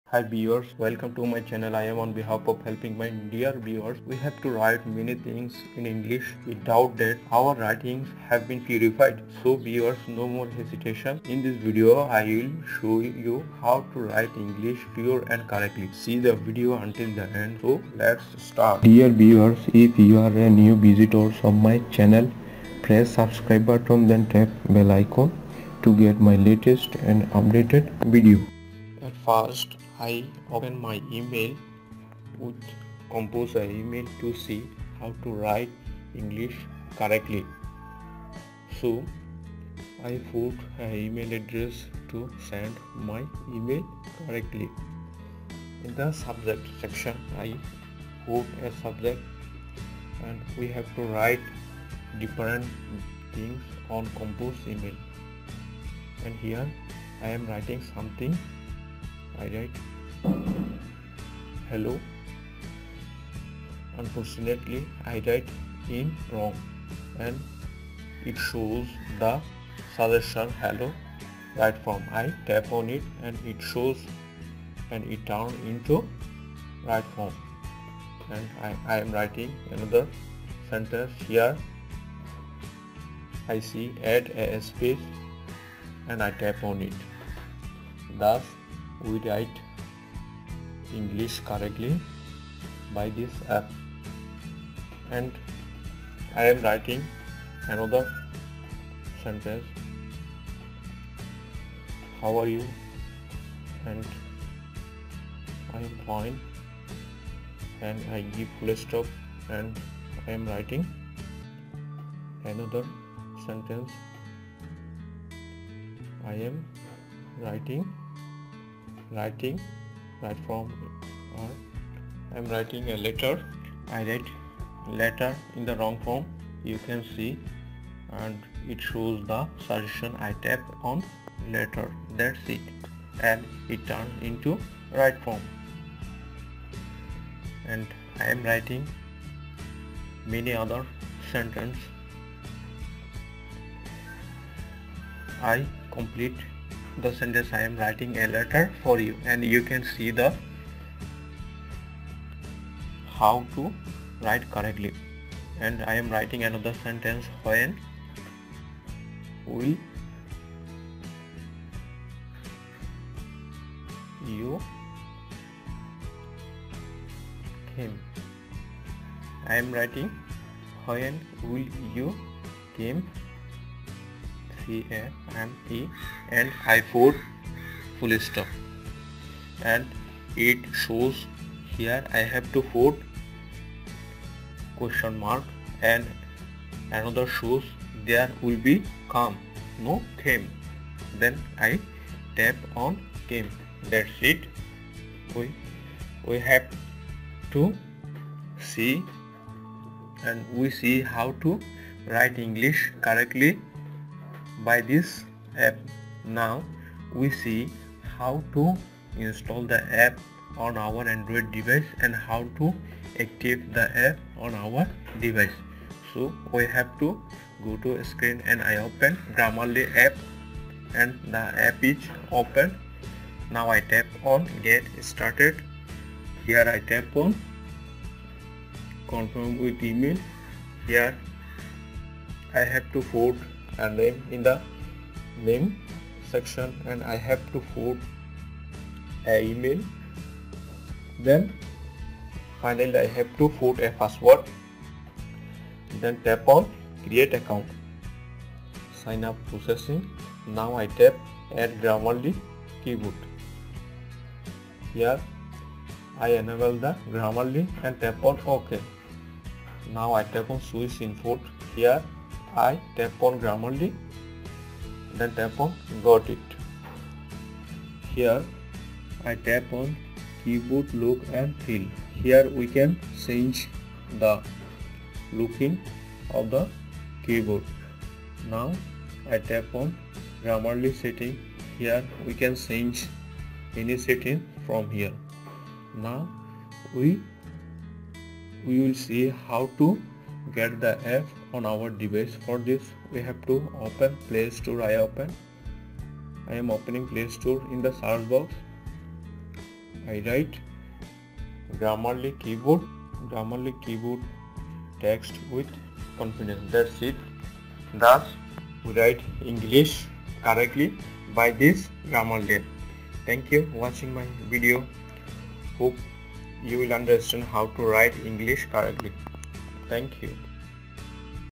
Gramerly Review Gramerly For Android English Correction App Gramerly Type With Confidence
This video covers the  Gramerly Review Gramerly For Android English Correction App Gramerly Type With Confidence

*Phone:
Samsung Galaxy J7 Prime

*Mouse:
HP x3000 Wireless Mouse

*Usb Cable:
BrexLink Micro USB

*Huawei Mate

*Micro USB

The Grammarly Keyboard helps you write mistake-free and with ease in any app you use on your mobile device. It provides hundreds of checks and features, along with seamless integration. Whether you’re sending an urgent email, important LinkedIn message, or essential Facebook post, you can write from your phone with confidence.

Mistake-Free Writing
– Sophisticated grammar checker
– Contextual spelling checker
– Advanced punctuation correction
– Vocabulary enhancements

Works Everywhere
– Keyboard integrates smoothly with all apps

Improve Your Skills
– See short, clear explanations for every correction
– Helps you understand your mistakes and avoid them in the future